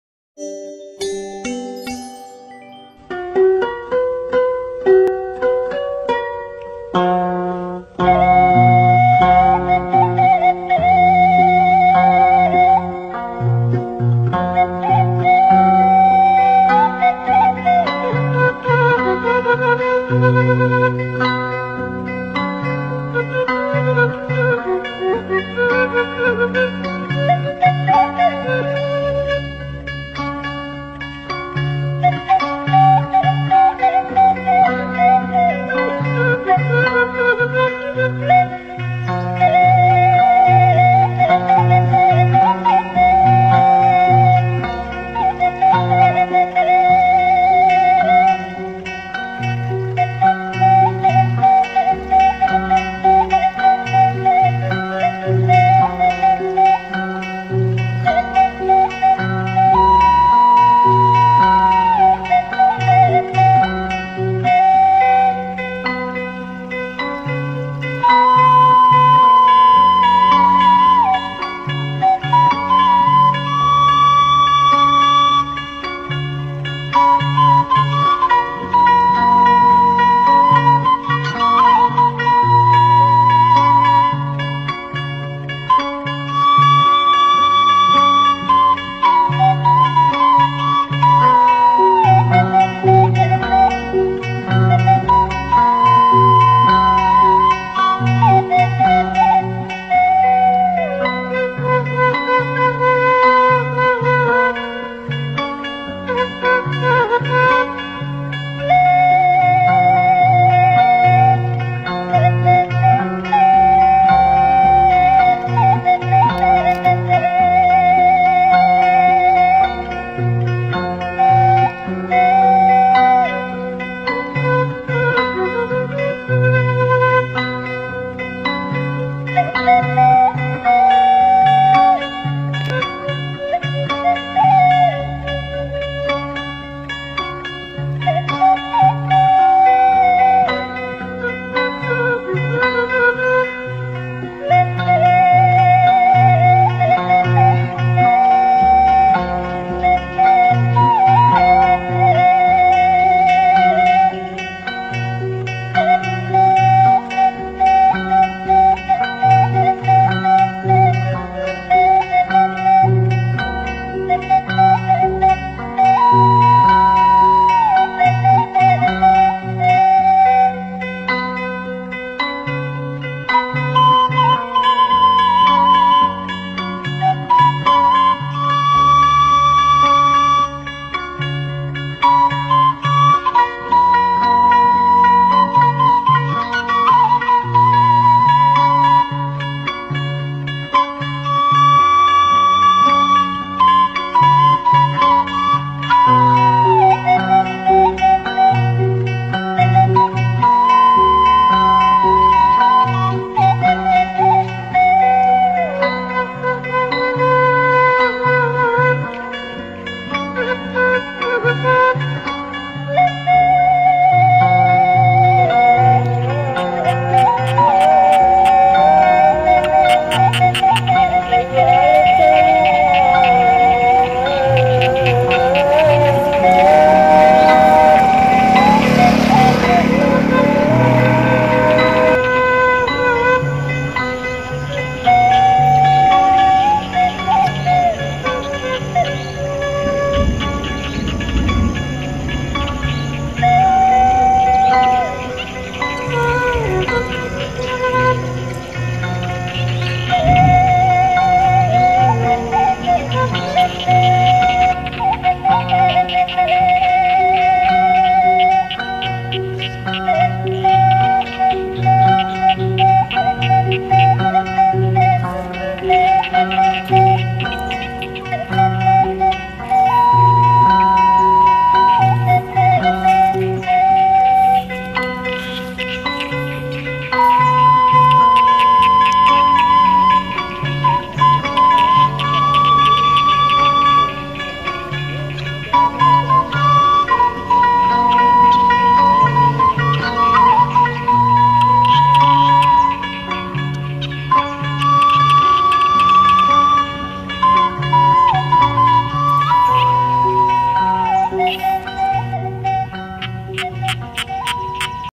Persawahan desa Sukamantri Cikarang//Senja di pematang sawah//nyanyian katak tak bernada//
Sore hari jalan jalan dipematang sawah didesa Sukamantri Cikarang Bekasi, menikmati suara nyanyian katak walau tanpa nada namun cukup memberikan ketenanga hati.